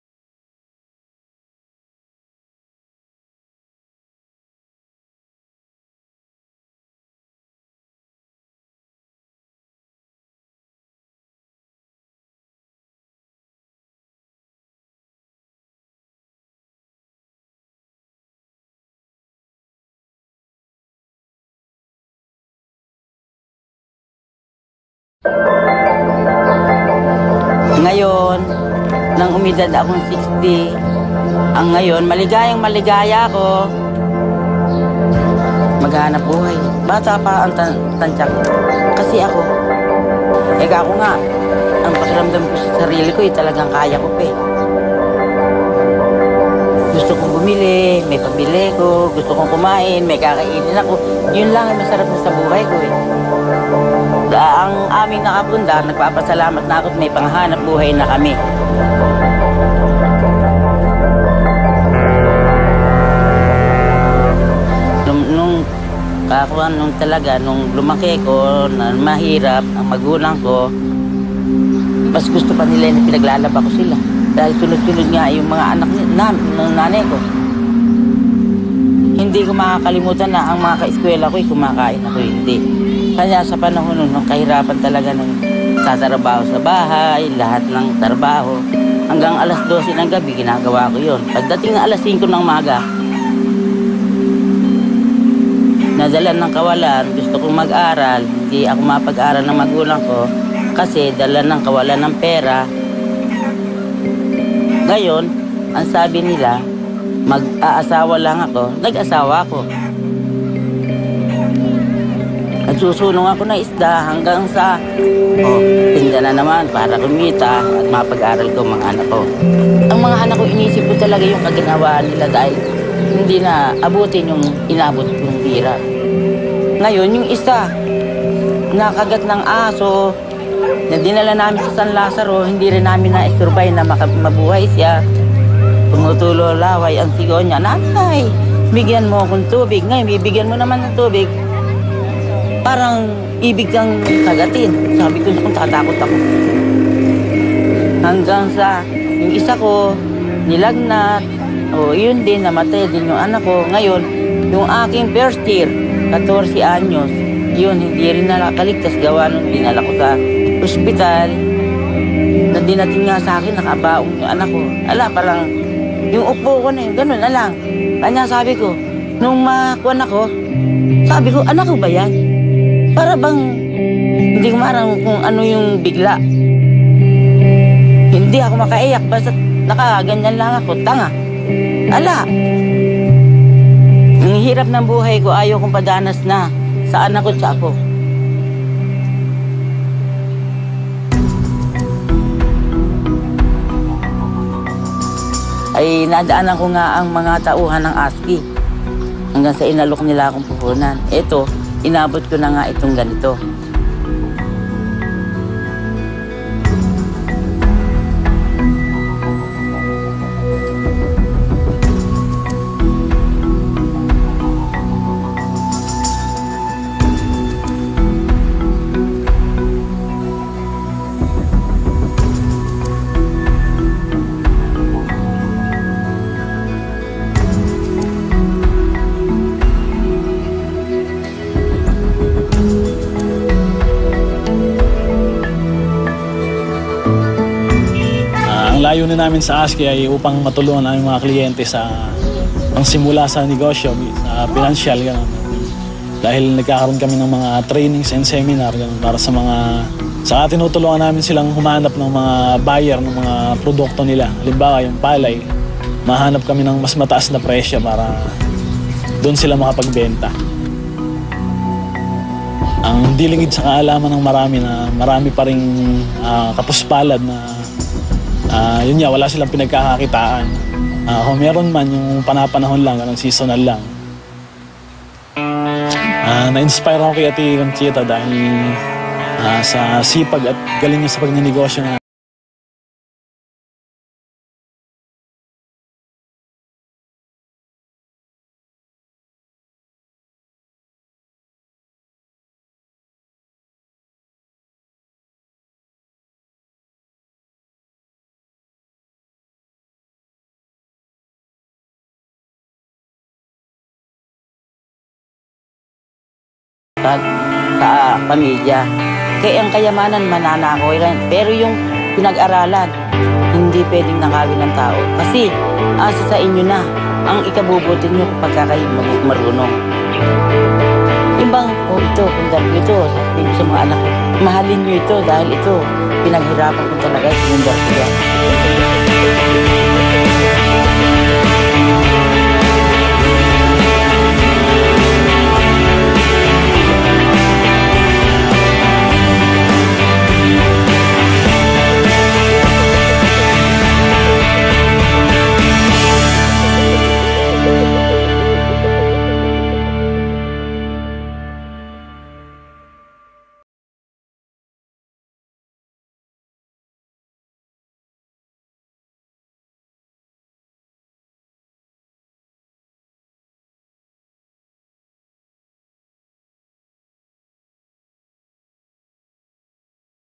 Historia de Conchita, una cliente de ASKI
De niña, creciendo en Luzón Central, Filipinas, Conchita no pudo terminar sus estudios. En lugar de eso, trabajaba en casa todo el día lavando ropa. Conchita prometió que ayudaría a sus hijos. Cuando Conchita se reunió por primera vez con empleados de Alalay Sa Kaunlaran, Inc. (ASKI), un proveedor de microfinanzas en Filipinas, ella y sus hijos vivían con menos de US$1,25 al día.
Este video fue amablemente proporcionado por ASKI y Opportunity International Australia.